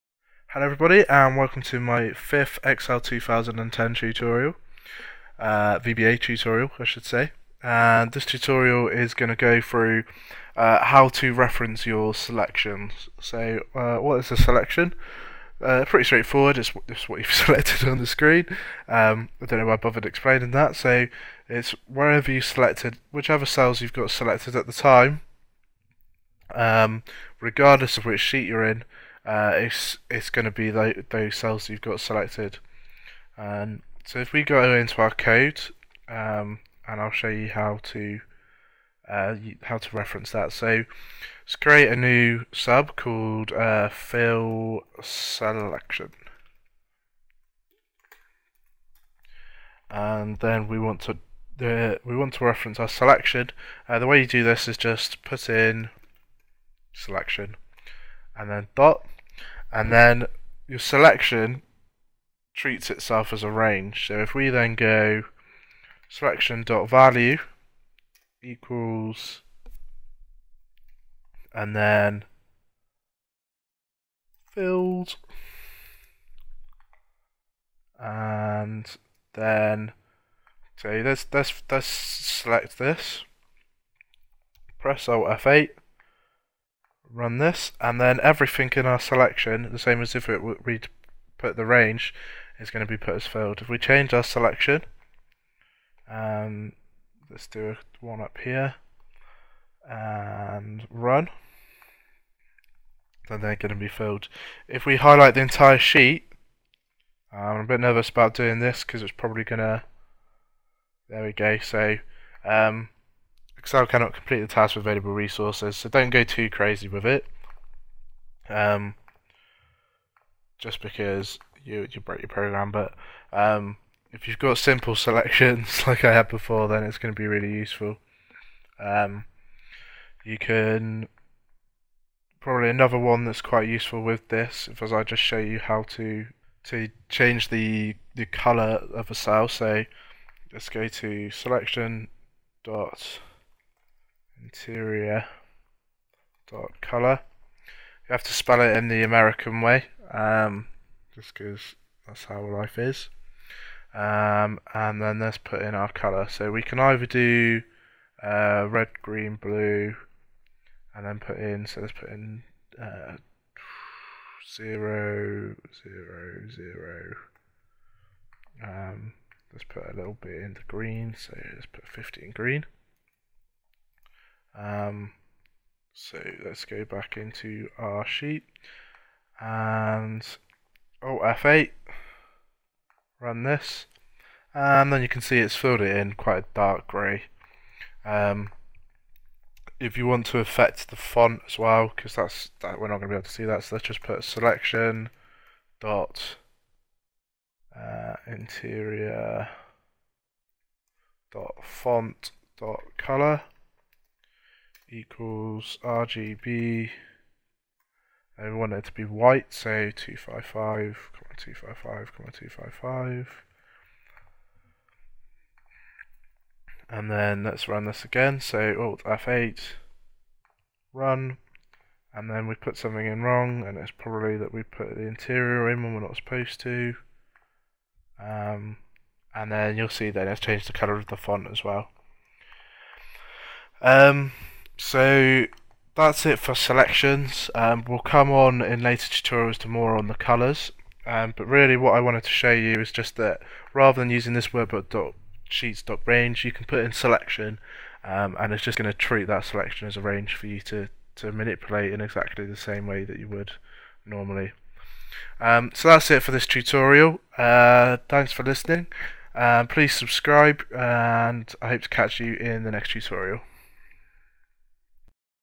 Excel 2010 VBA Tutorial 5 - Referencing Selections
How to reference the currently selected cells.
Website and Forum - Please direct any questions you have here.